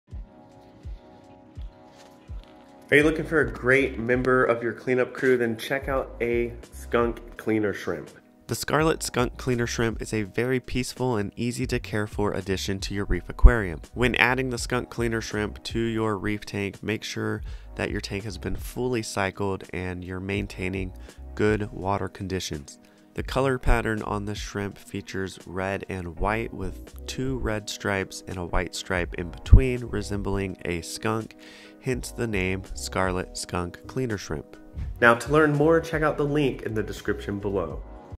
You NEED A Skunk Cleaner Shrimp For Your Reef Aquarium!
A skunk cleaner shrimp is an easy to care for member of the clean up crew that will help your reef aquarium thrive! You definitely need a cleaner shrimp.